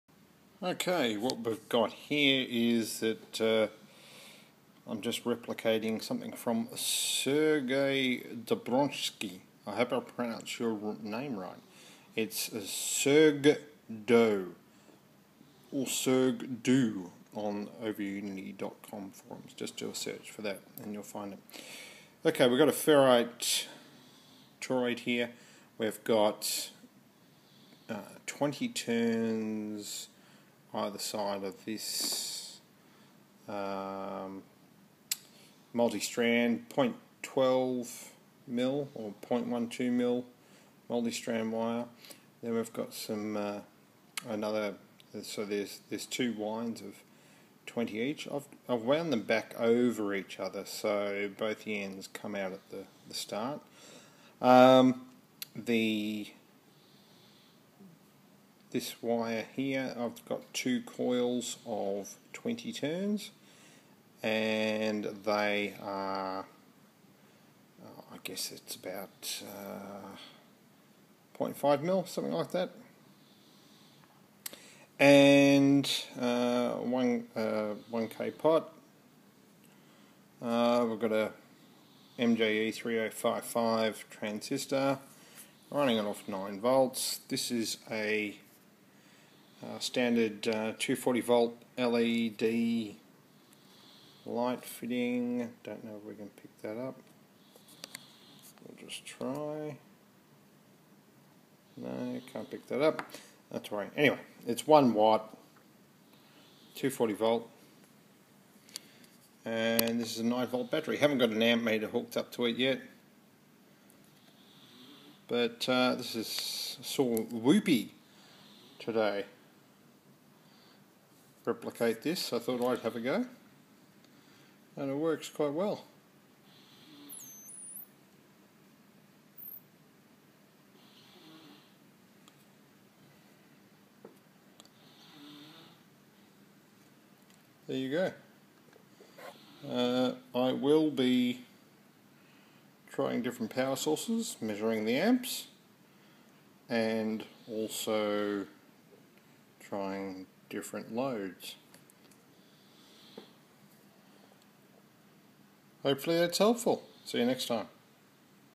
Blocking oscillator?
Ithink that is what sergei calls it. Not sure what I've built here. Lights a 240v led lamp rated at 1 watt. Search for sergdo on overunity.com for circuit. Really simple to make.